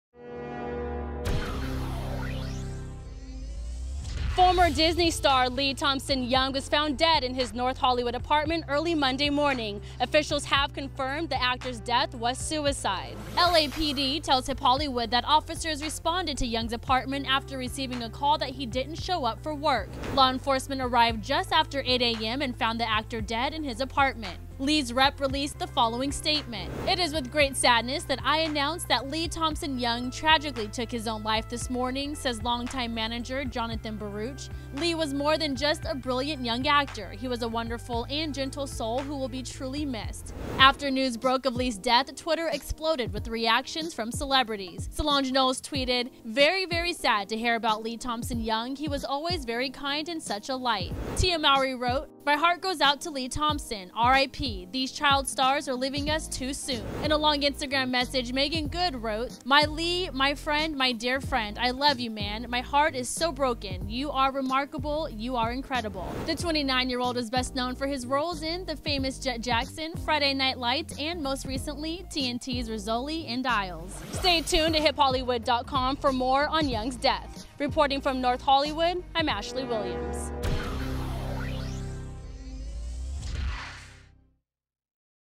Former Disney Star Lee Thompson Young Dead at 29 - HipHollywood.com
Former Disney star Lee Thompson Young killed himself early Monday morning in his North Hollywood apartment.